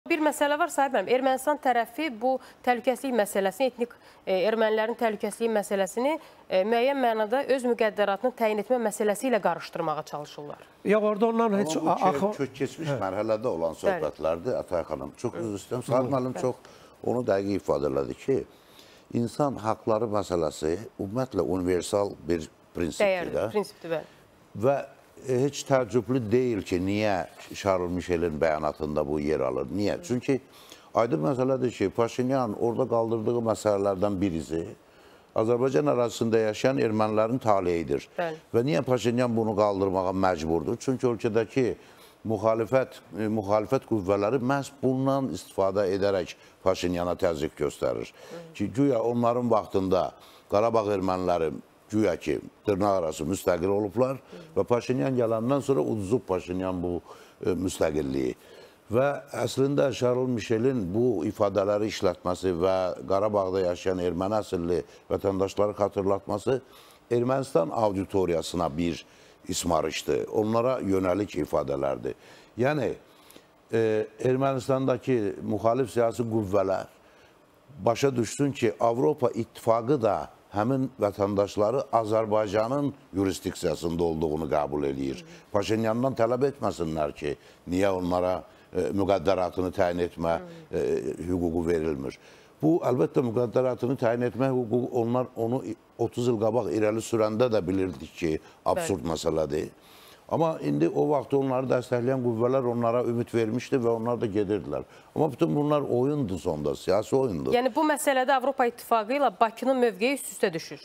Avropa İttifaqı Qarabağdakı erməni əhalini Azərbaycanın yurisdiksiyası daxilində qəbul edir
Aİ-NİN QARABAĞDAKI ERMƏNİLƏRLƏ BAĞLI MÜNASİBƏTİ